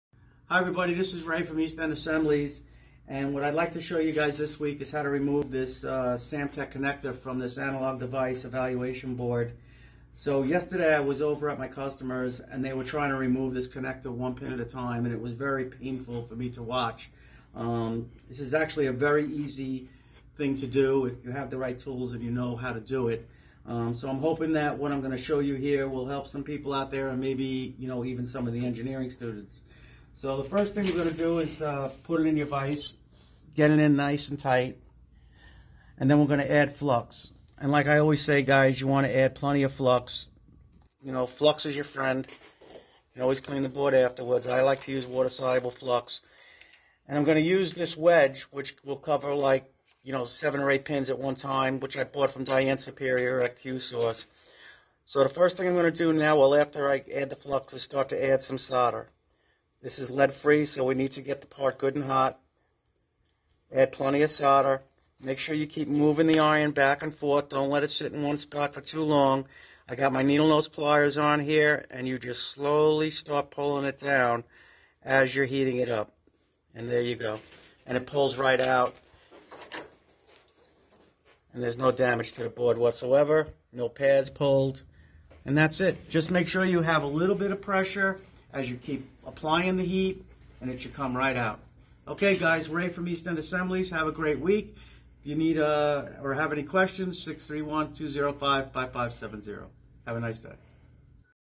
How To Remove a Samtech Connector
How To Remove a Samtech Connector with Ray Novara
A fast an easy approach that you can do.